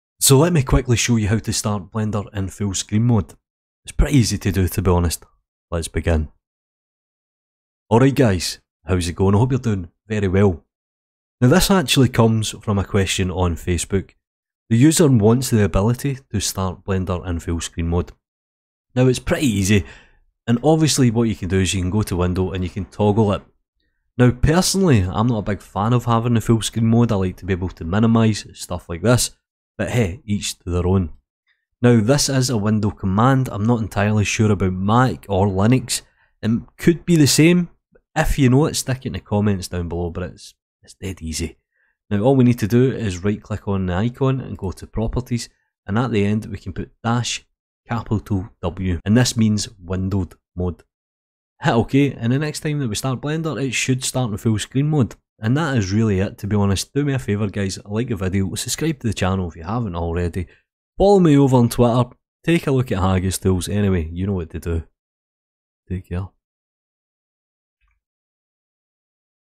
Start Blender in Full Screen mode -W
Sorry for the low production value, but this is a quick way to start Blender in fullscreen mode. use the command -W